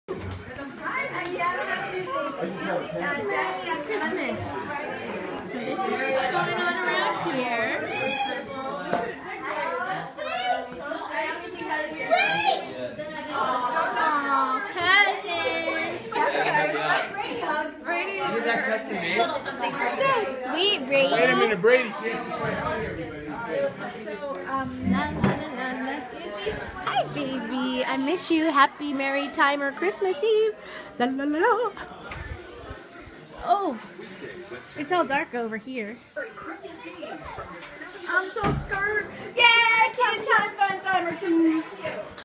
Christmas Eve 2015
Christmas Eve at home with family. My dear hearts.